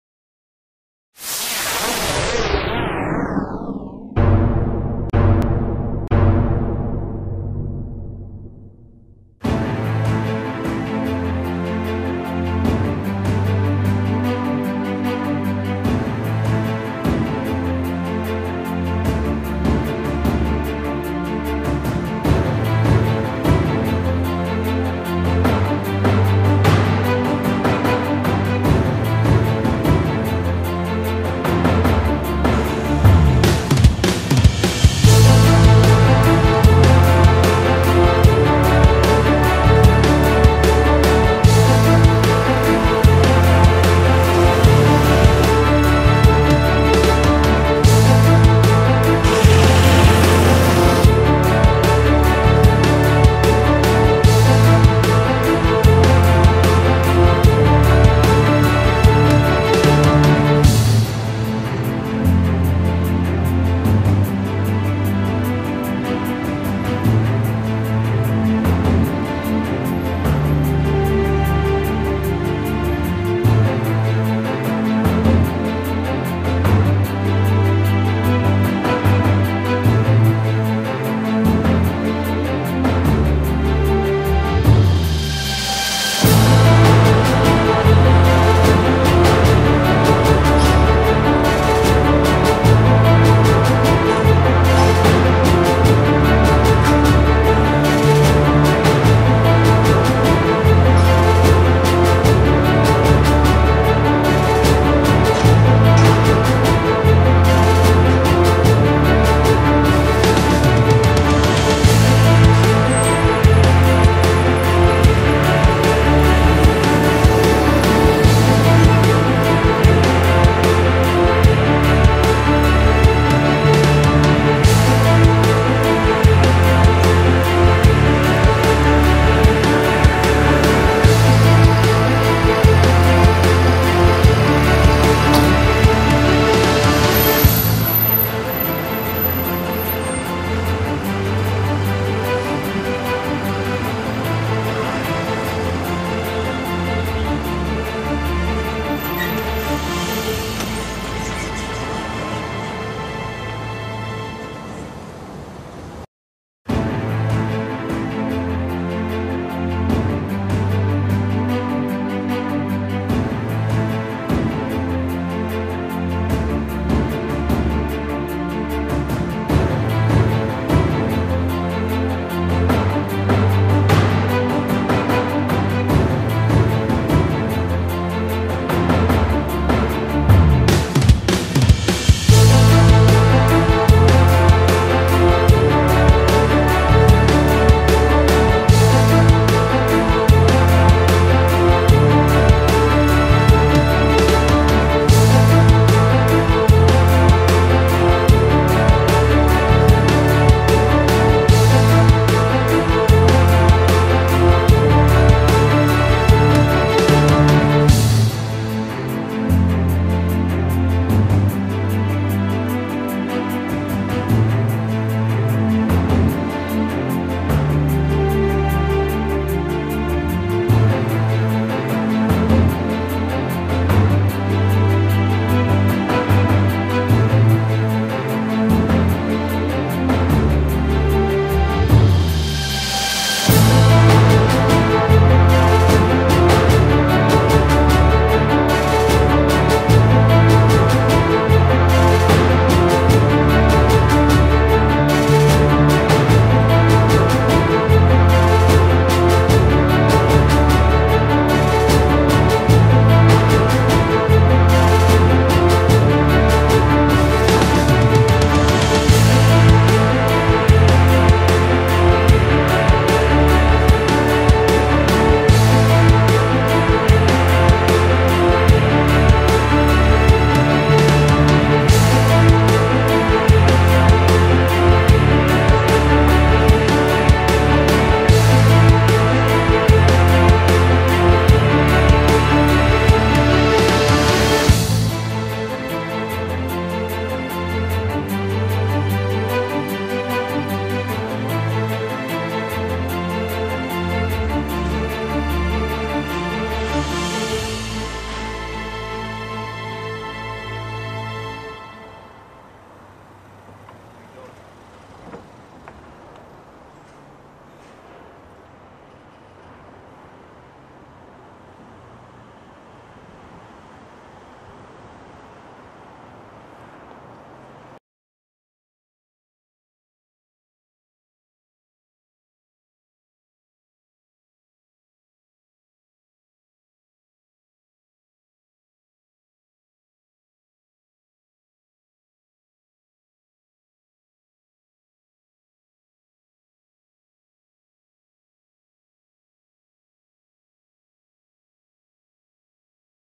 HySecurity Parking Gate Operators
HySecurity Parking, Traffic Control and Security barrier arms and wedge operators shown on location. Experience HySecurity's StrongArmPark DC traffic control, parking and revenue control battery backed up, smooth cycling, barrier arm. HySecurity's vehicle theft protecting WedgeSmart DC has virtually eliminated rental car agency vehicle thefts at Seattle's SeaTac Airport Consolidated Parking and early Avis and Hertz Rental Car agency locations.